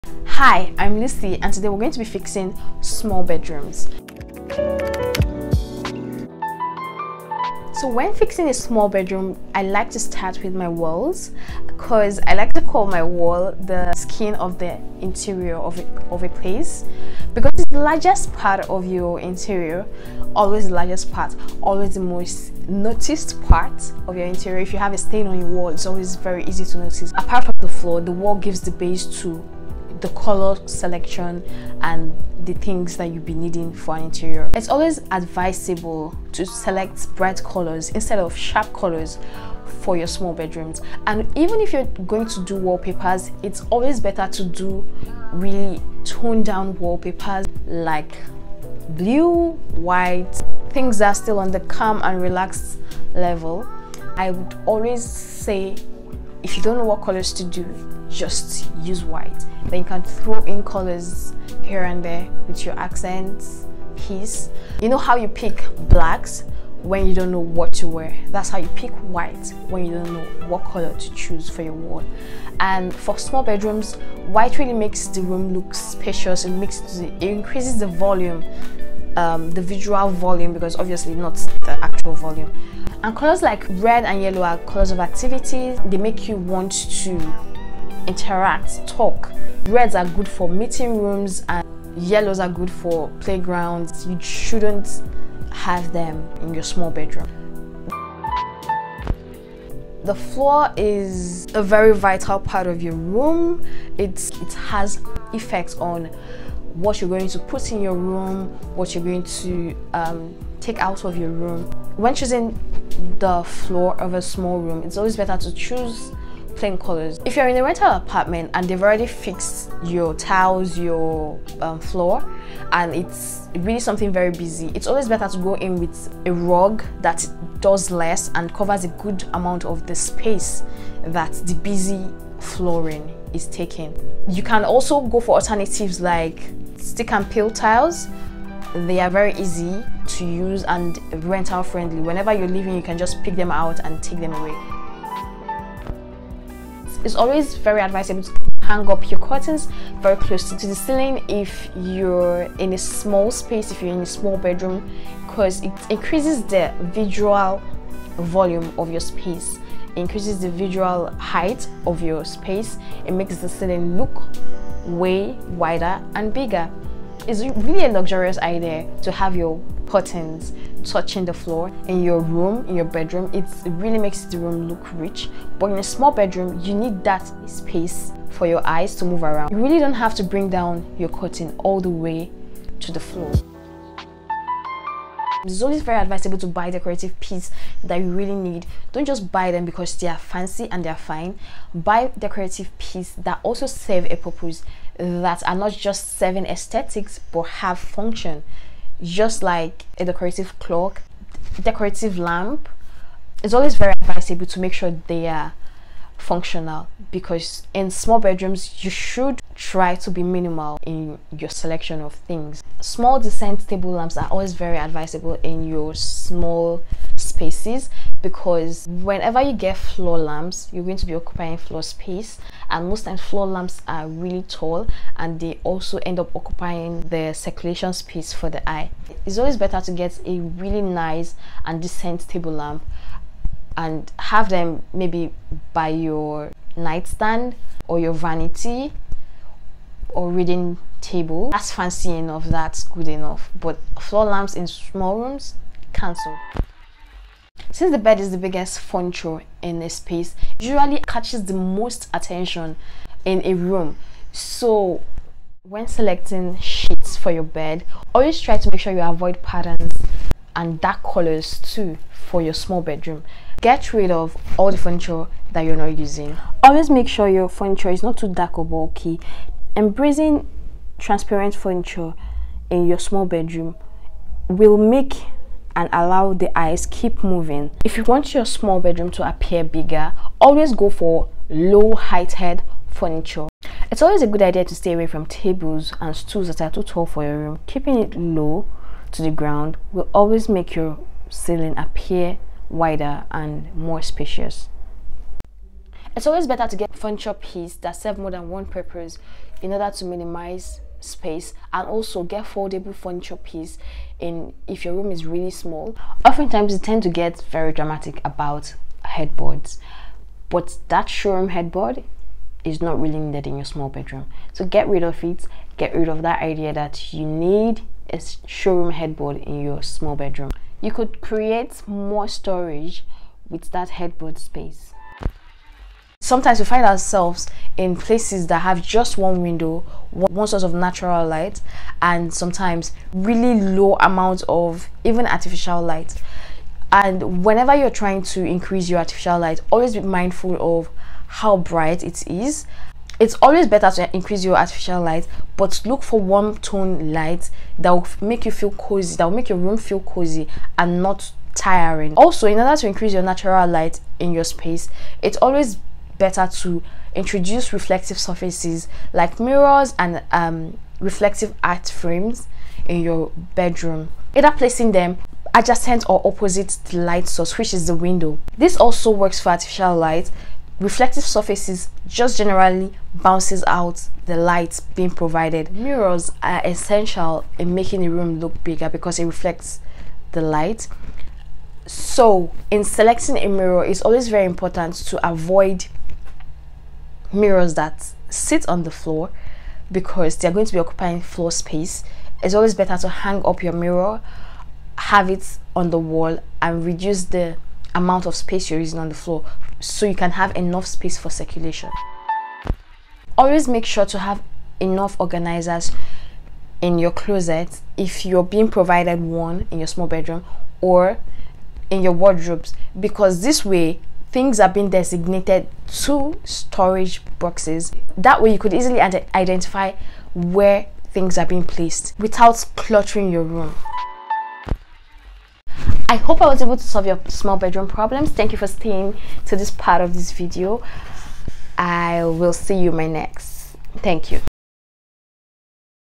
How to Make a Small Bedroom Look Bigger
Small bed room makeover over can be difficult sometimes if the right principles of designs are not been followed. In this video I listed few ways you can make your small bedroom a lot more comfortable.